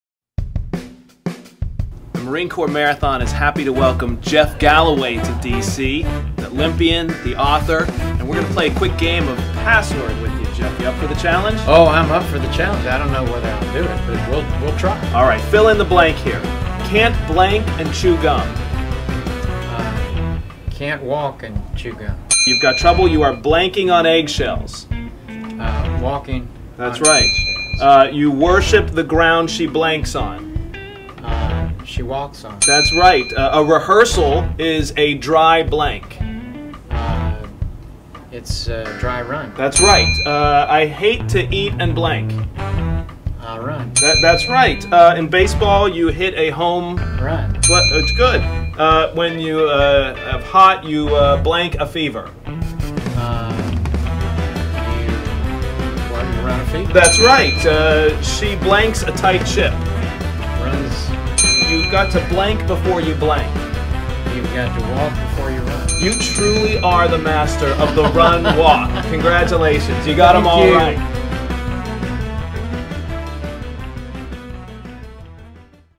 Quizzing Jeff Galloway
It's game show time with the "Run-Walk" guru in celebration of his selection to the MCM Hall of Fame.